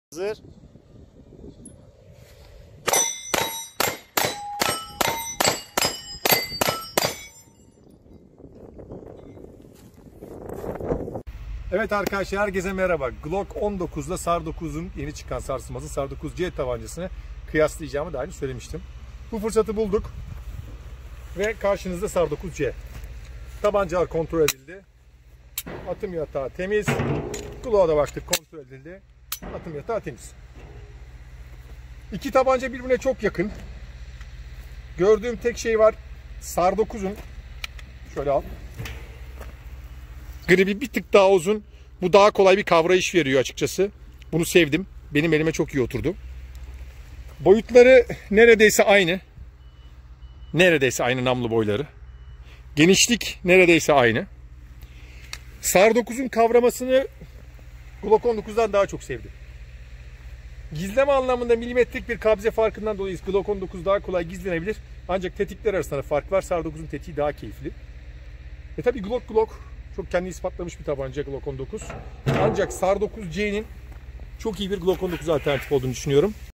Sar9c - Glock19 boyut kıyaslaması
@sarsilmazsilah Sar9c ile glock 19... Tabanca secimi kisiye gore cok degiskendir, video sadece tabanca boyutlari ile ilgilidir ve benim hissiyatimi yansitir... ulkemize Glock tabanca gelmedigini dusundugumuzde, sar9C'nin cok iyi bir alternatif oldugunu dusunuyorum. Elbette tetigi Glock tetigi ile kiyasladim. Bu arada sar9c tetigi kanaatimce sar9'dan daha guzel bir tetik👍 LUTFEN benden baska Turk markalarimiz ile kiyaslama yapmami iatemeyin ve sormayin.
Iyi is yapqn Turk silahlarini hep destek tam destek🇹🇷